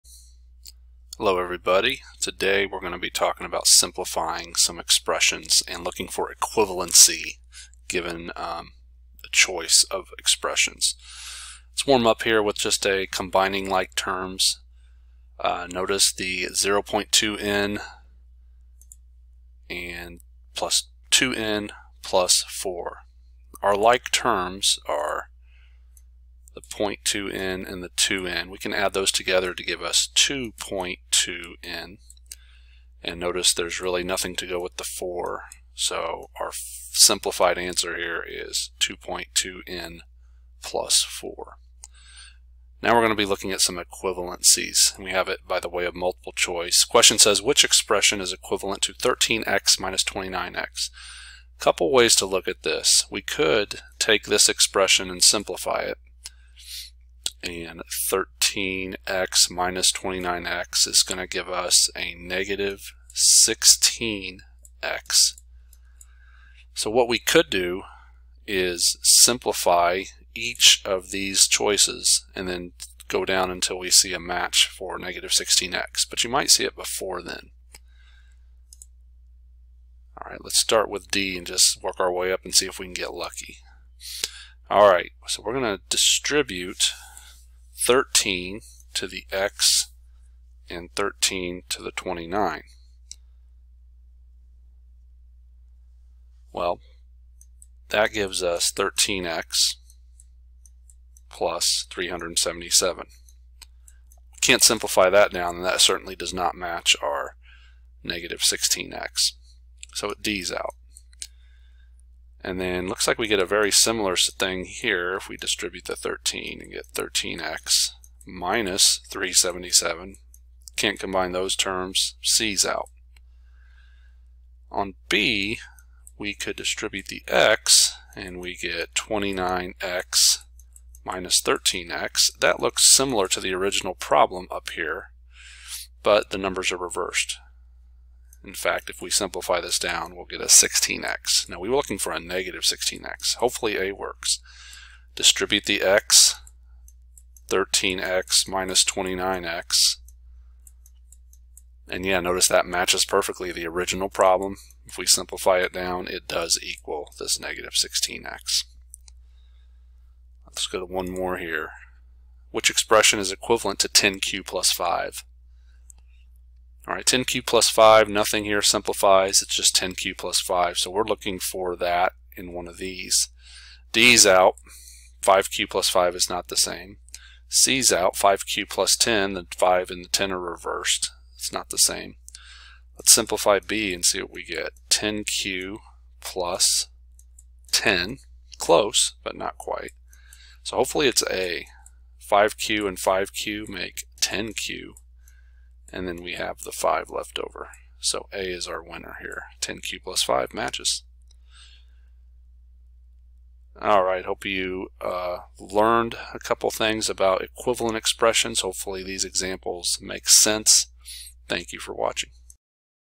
Matching Equivalent Expressions
1 example of simplifying and 2 examples of matching equivalent expressions